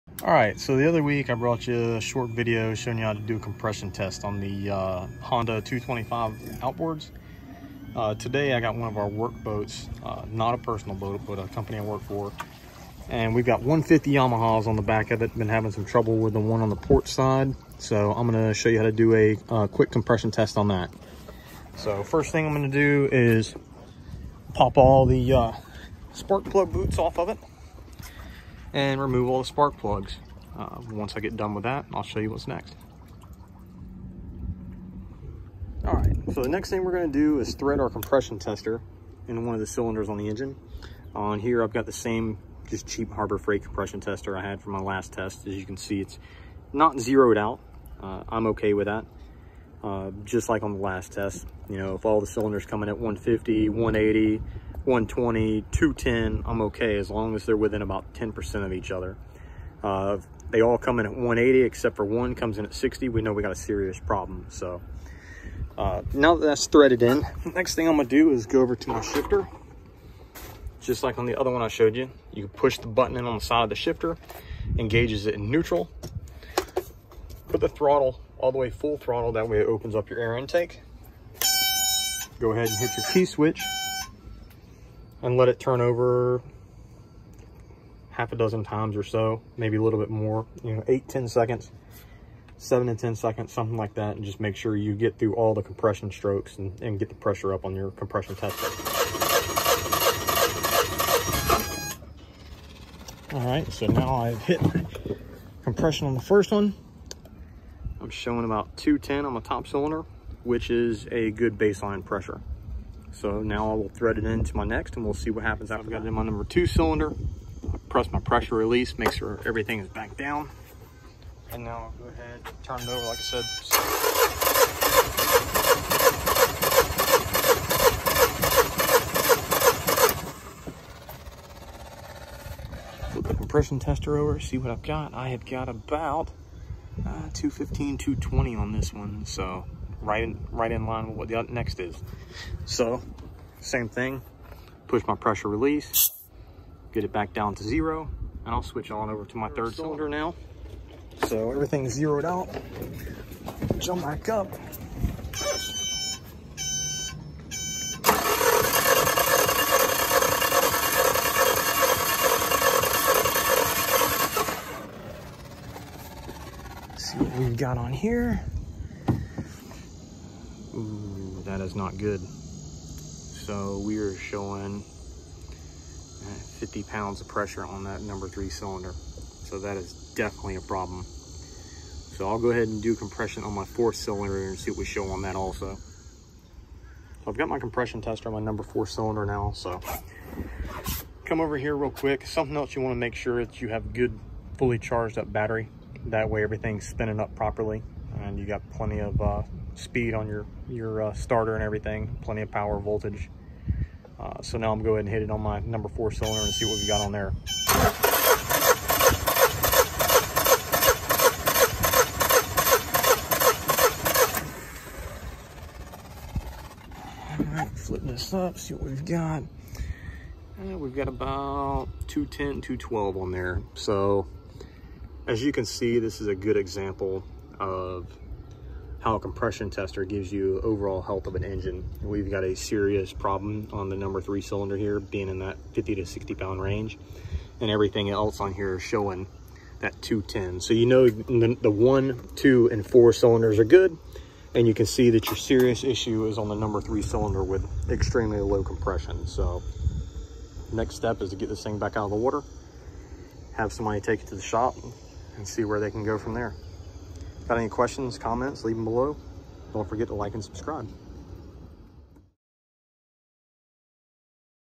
Yamaha Outboard Compression Test How To and Failure
How to compression test a Yamaha Outboard. This engine failed the test and upon further inspection the engine has burned a piston.